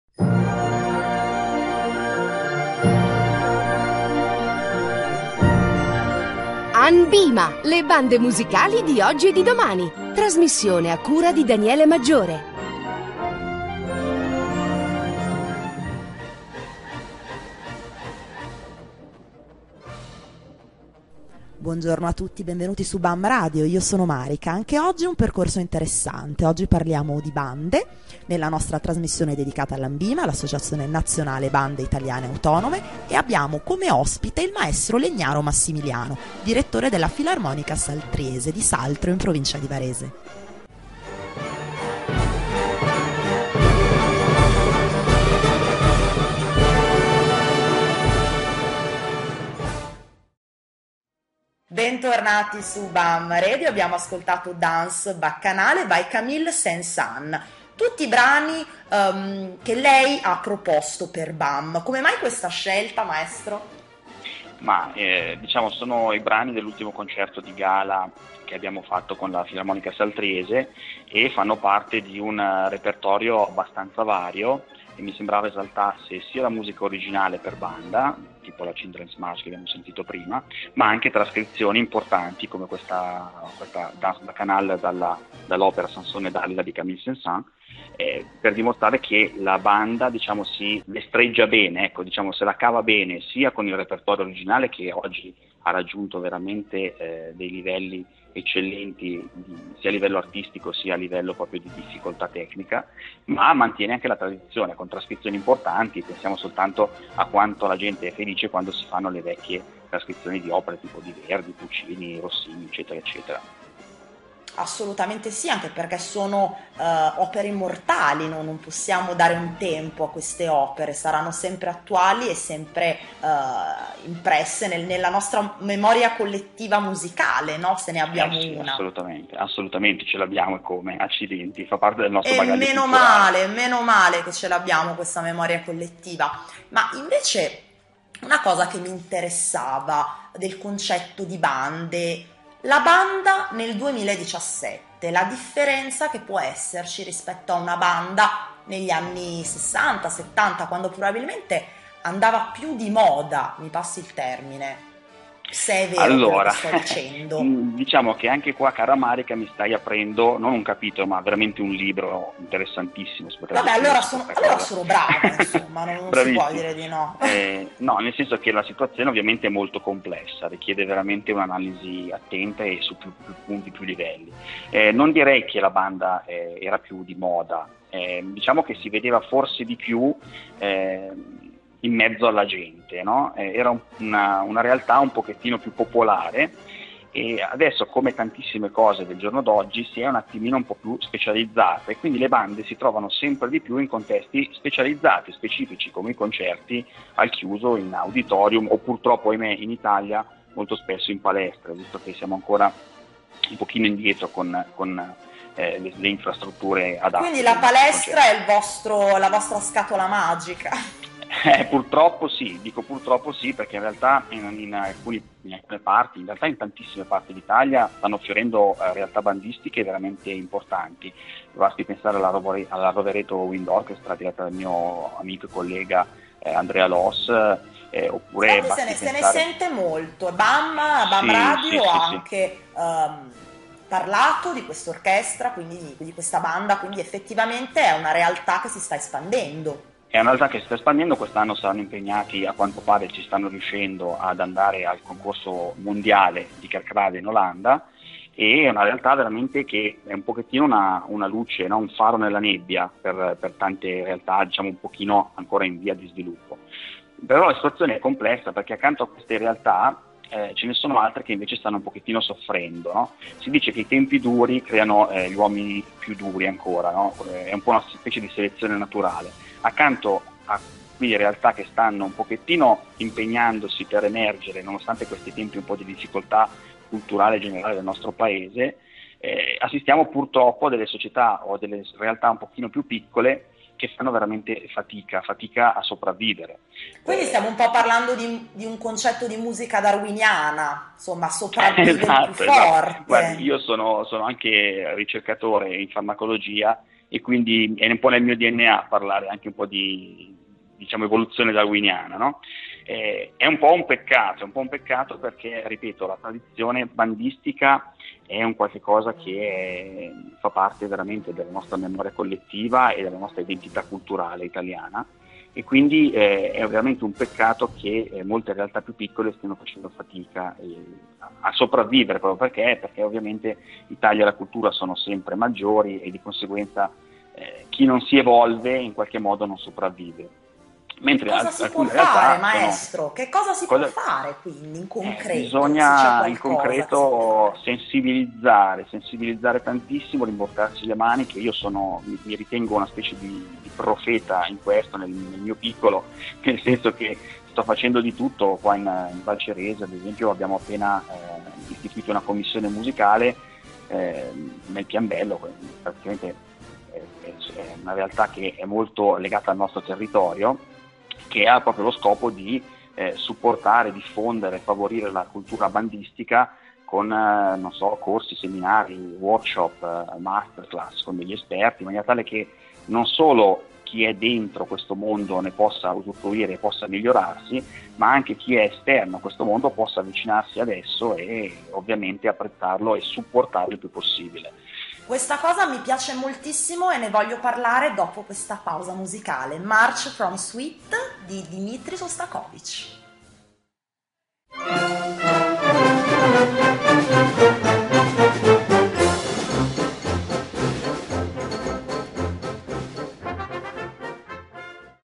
Intervista Direttore Filarmonica Saltriese pt3
Intervista al Direttore Artistico della Filarmonica Saltriese su Bam Radio